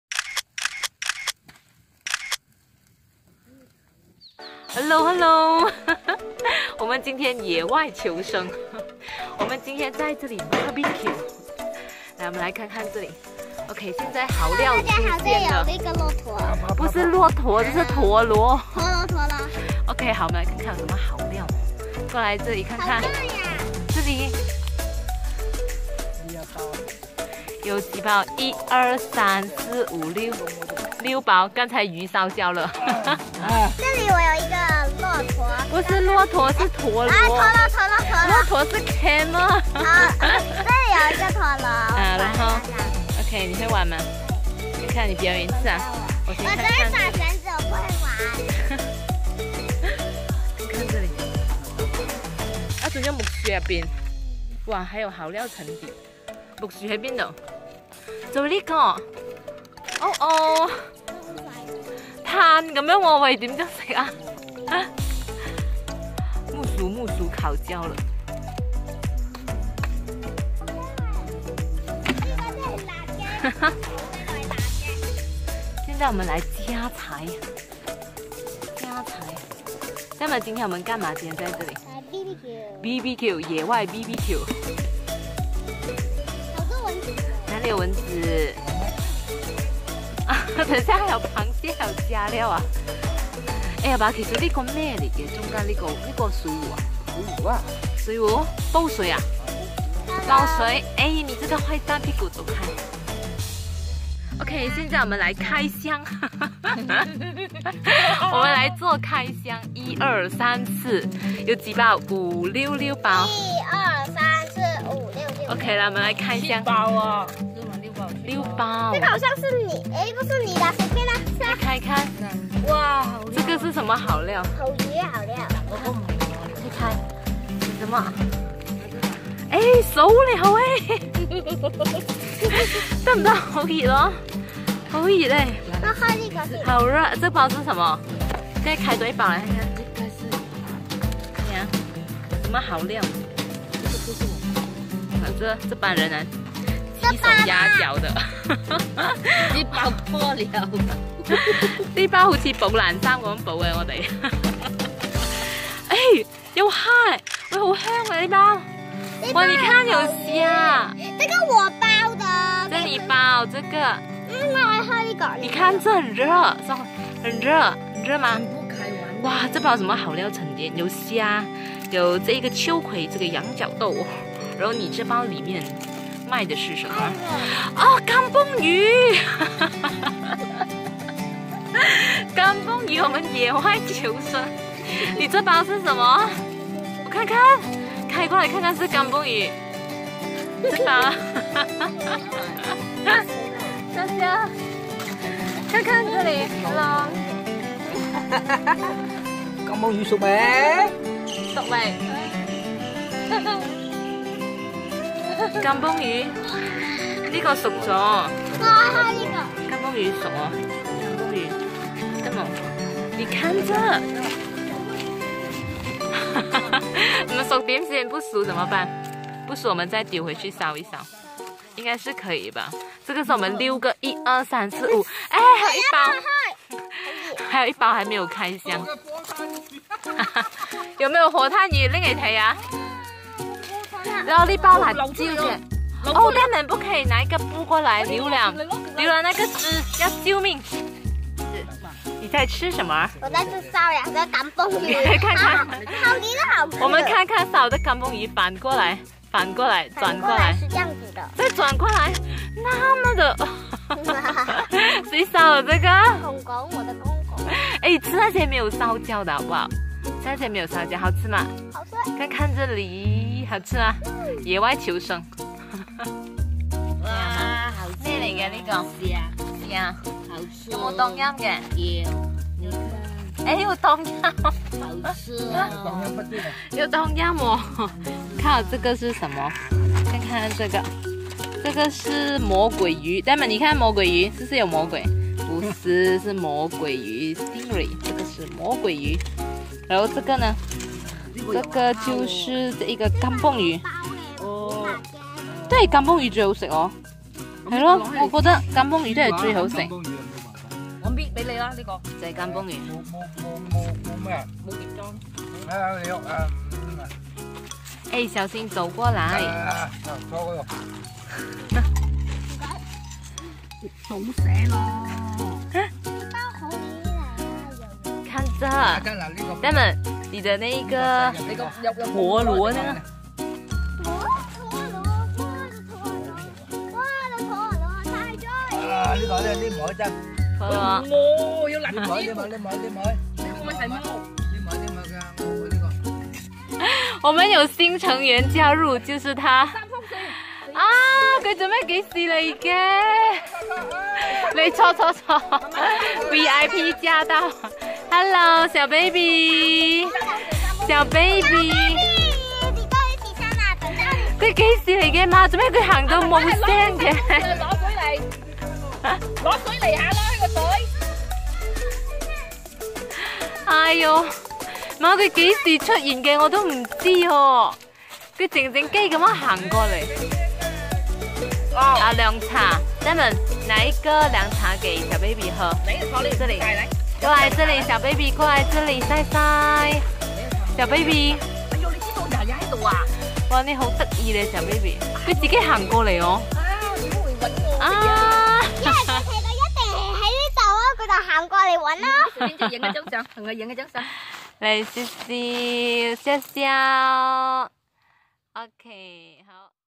周末野外求生记!! 烤焦的Kampung鱼最好吃哦~
Whereisteachergoing